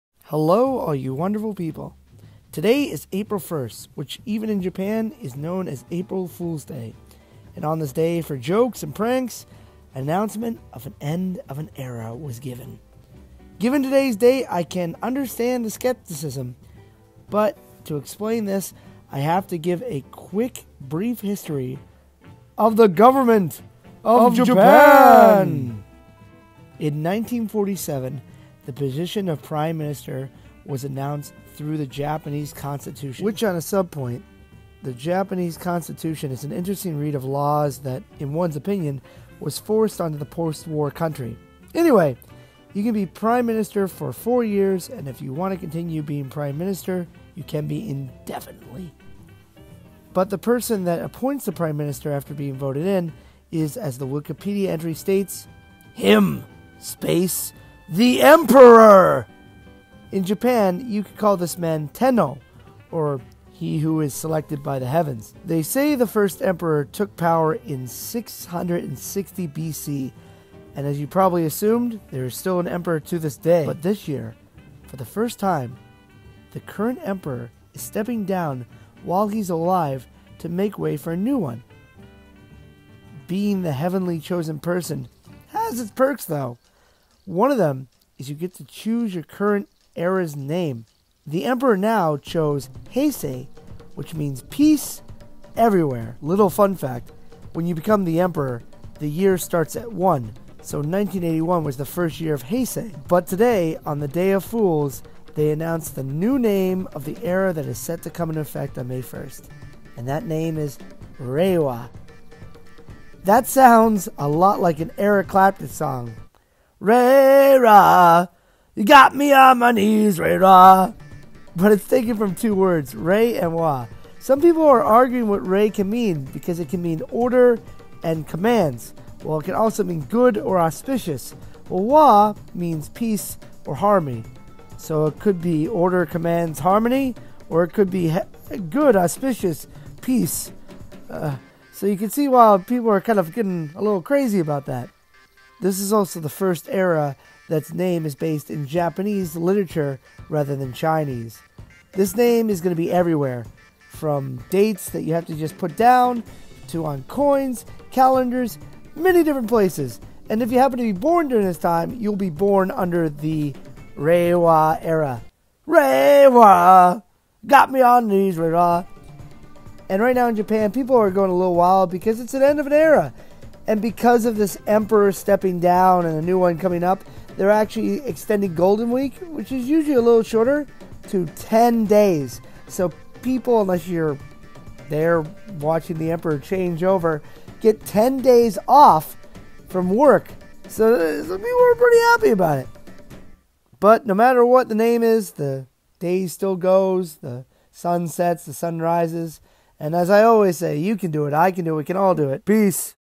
A new Era in Japan. What does Reiwa mean?
Today Japan announced the name for the next era of its existence “Reiwa” what does this mean? Click to find out